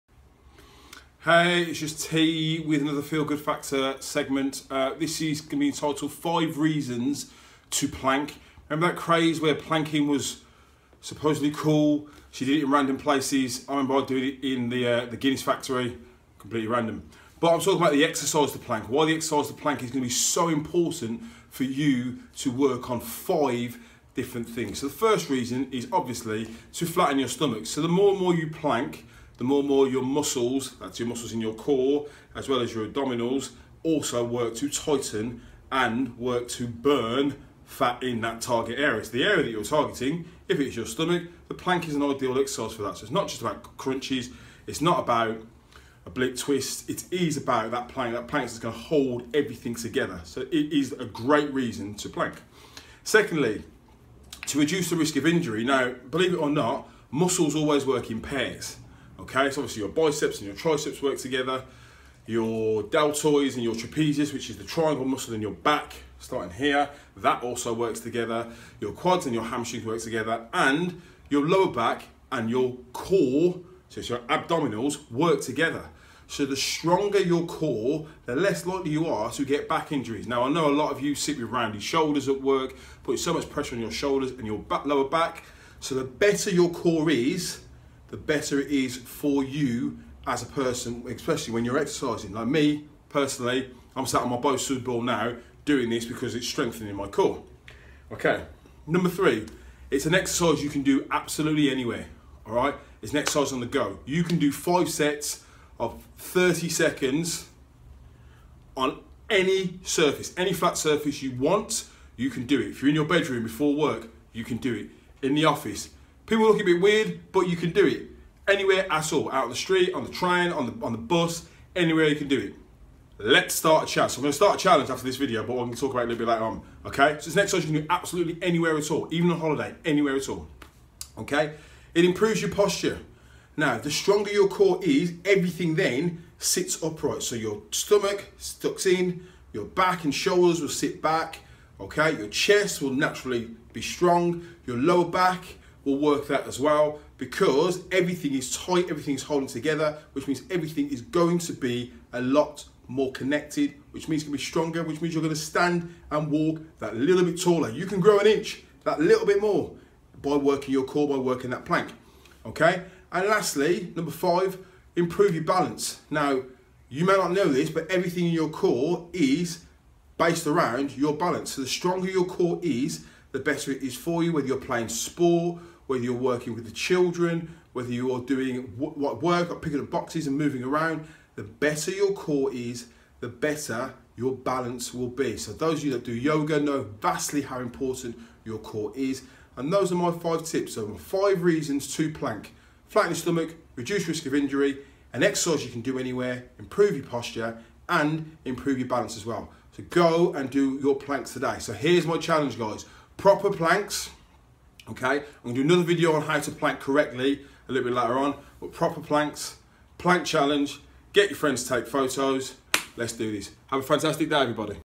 🔥5 REASONS TO PLANK🔥
Your core is the most important part of your body.

Health wise it allows you to;

💪🏻Flatten your stomach
💪🏼Reduce your risk of injury
💪🏽Exercise that you can do anywhere
💪🏾Improve your posture
💪🏿Improve your balance

Your muscles act together to maintain your body's structure and health.

Make sure you respect your core.

Start small.
20-30 seconds,
Build it up over time.
Trust your body and push yourself.